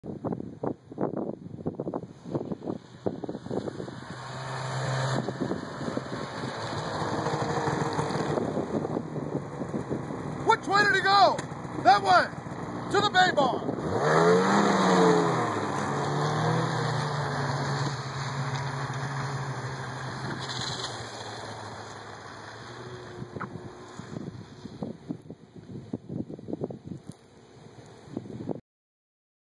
Radioactive Man Snowmobile - Bergland , MI - Follow the Journey!
Radioactive Man fighting evil cancer on his snowmobile, headed to the Bergland Bay Bar.